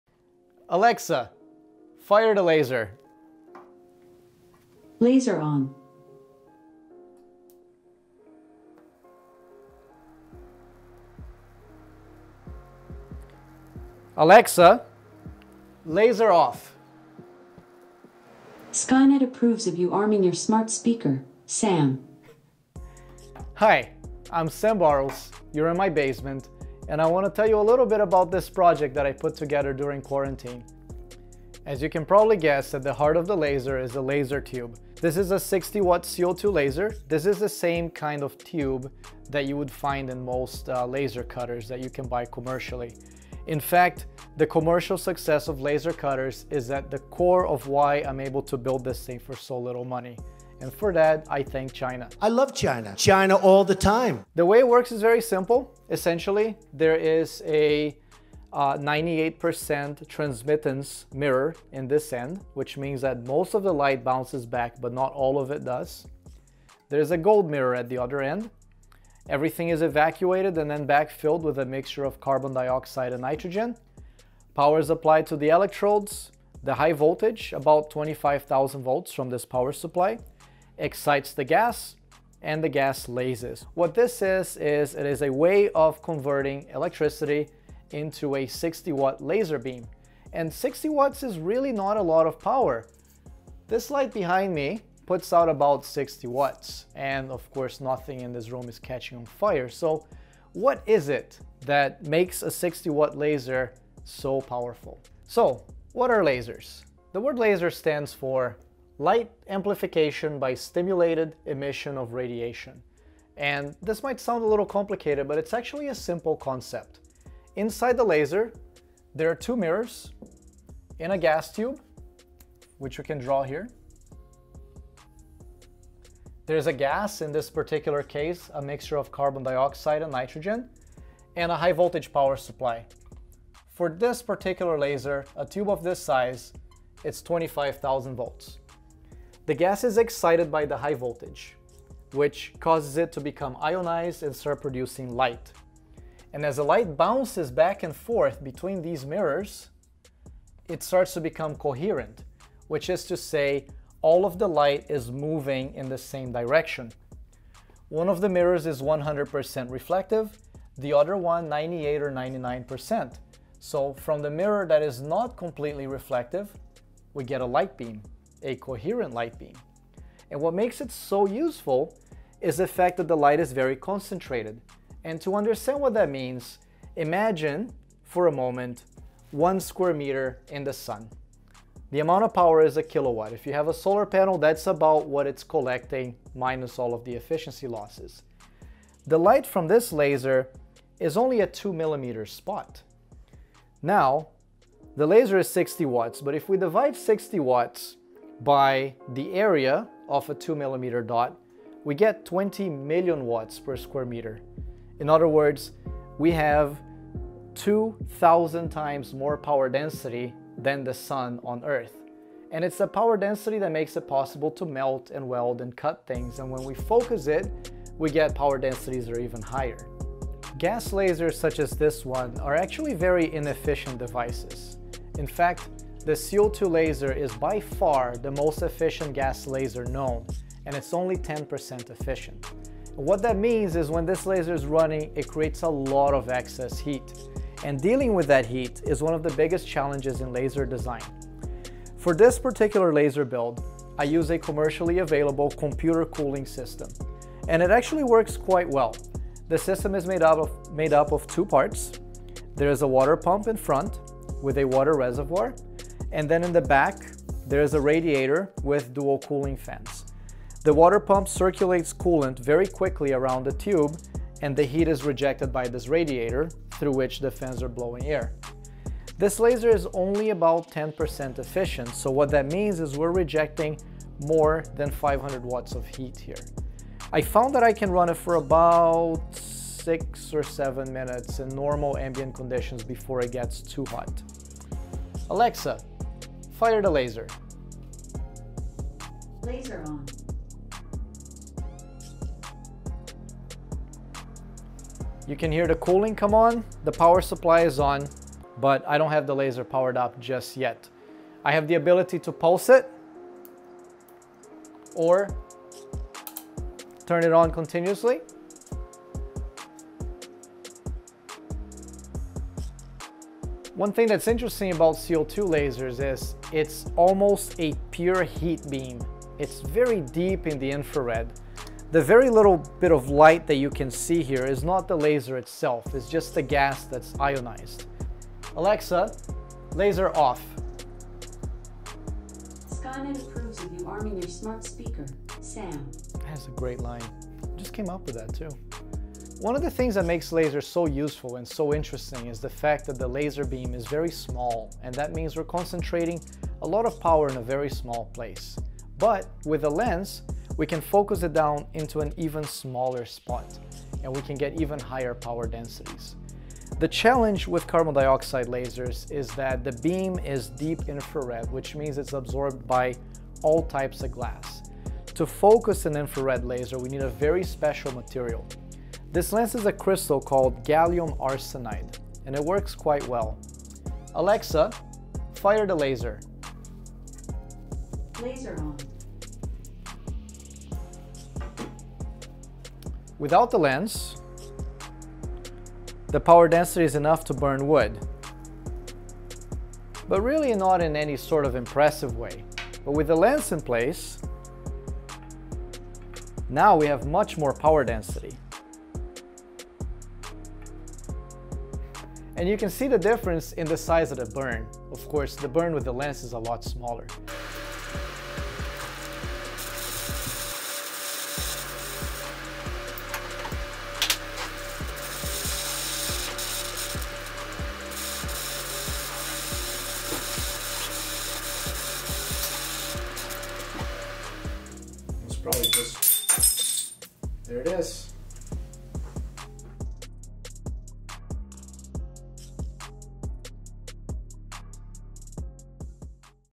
Home built 60W Burning Laser project
This was my first project when CoVid hit and the world went into quarentine; I went down to my basement and put together a 60W CO2 laser tube as a tabletop working laser complete with liquid cooling and smart speaker integration because, why not. I still don't know what to do with it, so here's a video showing it in action.